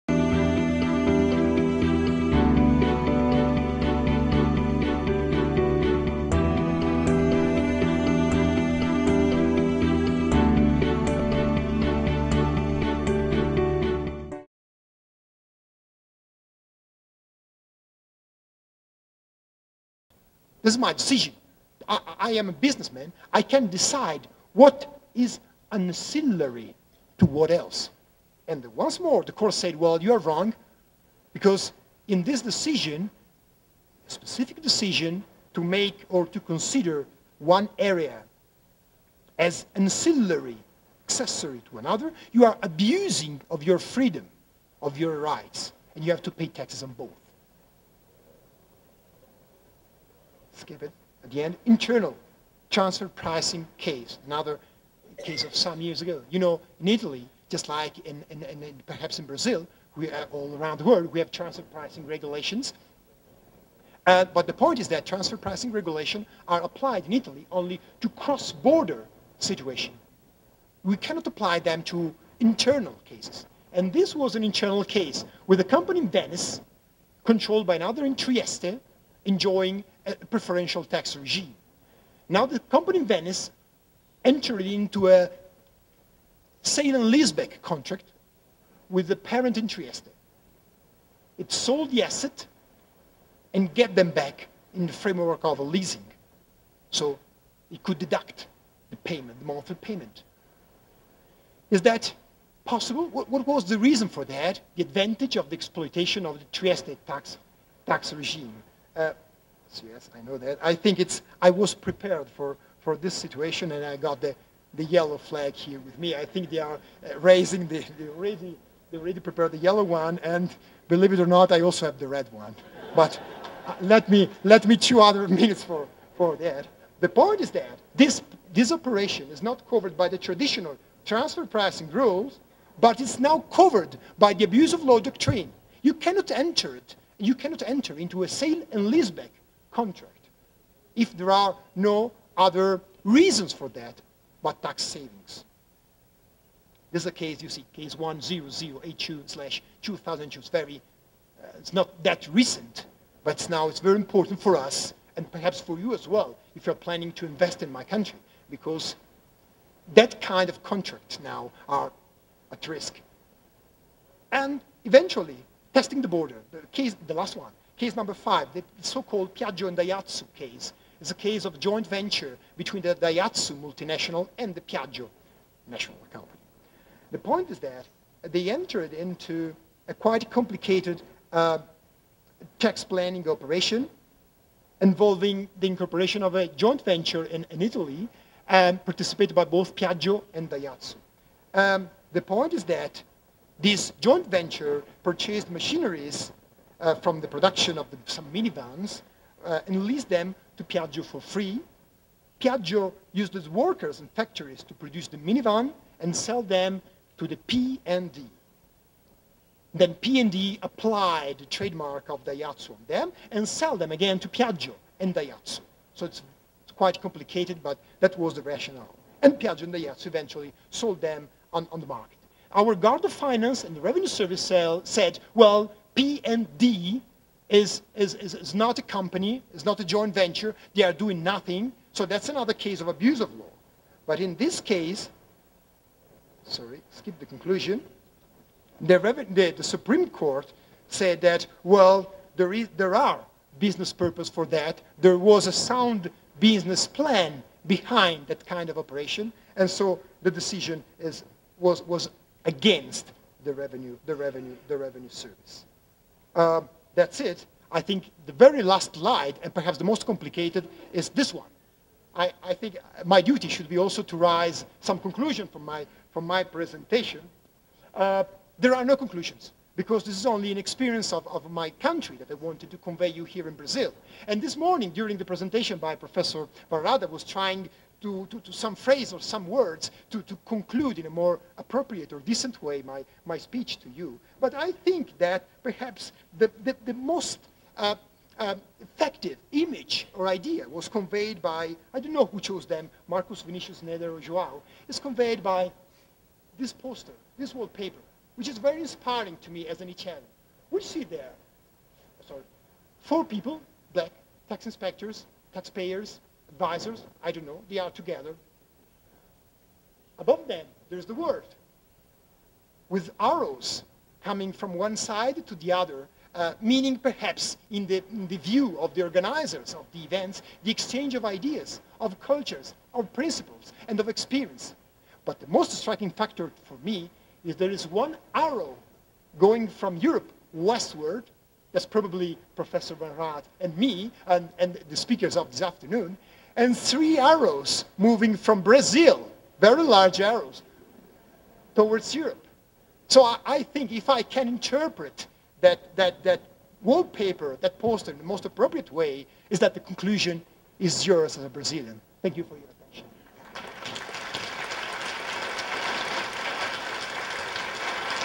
SSRNGA - Marcos Greggi - parte 5 de 5.mov
Evento: Seminário Sobre Regulamentação da Norma Geral Antielisiva
Painel: Norma geral antielisiva e o tratamento da falta de propósito negocial, da falta de substância e do abuso de forma nos negócios jurídicos. (A experiência Européia e a Italiana)
Palestrante: Marcos Greggi -- Universidade de Ferrara, Itália)
Data: 05/10/2010